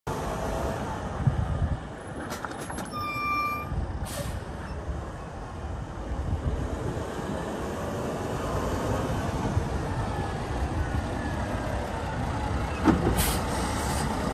2015 INTERNATIONAL PROSTAR For Sale
Navistar N13 6 Cylinder 12.4 L Ultra Low Sulfur Diesel, 450 HP Engine, 281,000 actual miles, 2014 US EPA Label, Requires Diesel Exhaust Fluid, 9892 Engine Hours, Jake, Allison Automatic Transmission, 12350# Front Axle, 40000# Rears, 52350 GVWR, Air Ride Cab, 11R22.5, Air Ride Suspension, 175" Wheel Base, Air Sliding 5th Wheel, PTO Applicable, Professionally Maintained Fleet Vehicle